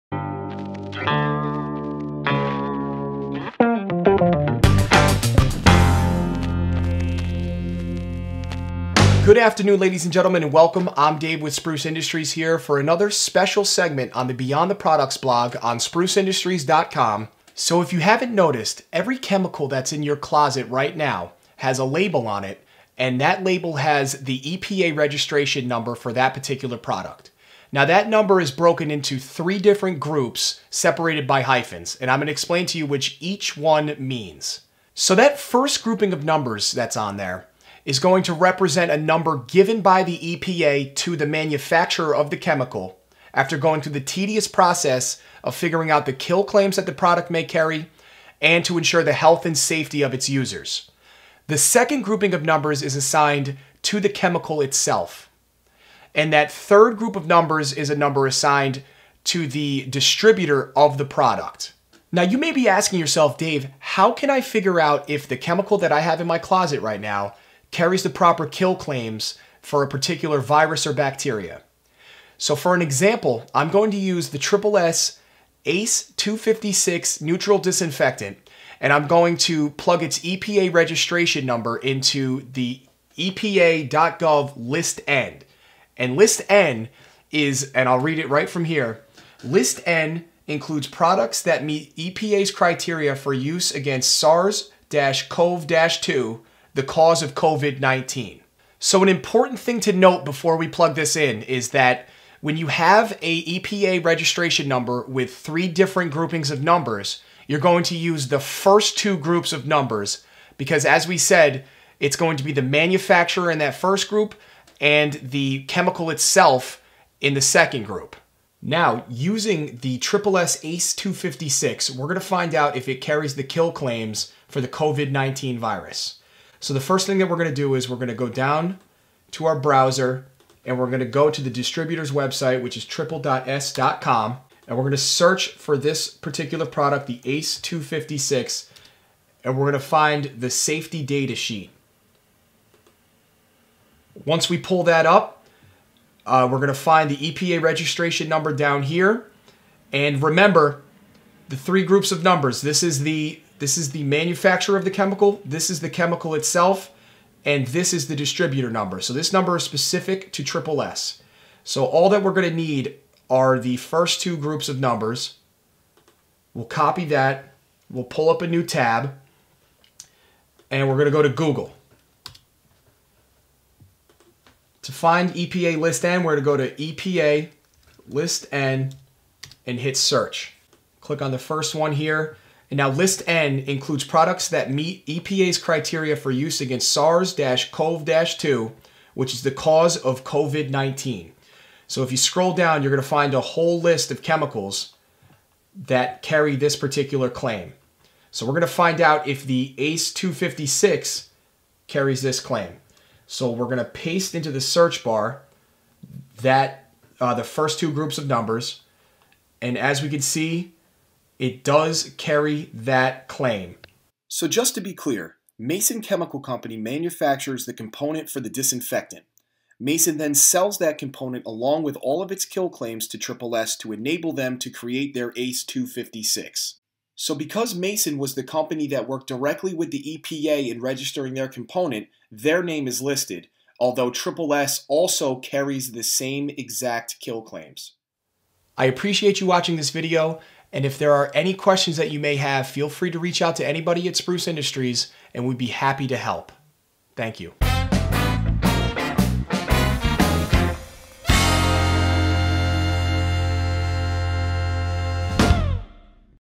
EPA Registration Number look up- Does a chemical carry the proper kill claims against COVID-19?
Those EPA Registration numbers listed on chemical labels can be confusing. Here's a short segment to describe what each group of numbers mean and how you can determine if what you are using, carries the proper kill claims against certain viruses. In this example, we tested the Triple-S Ace 256 neutral disinfectant with the COVID-19 virus.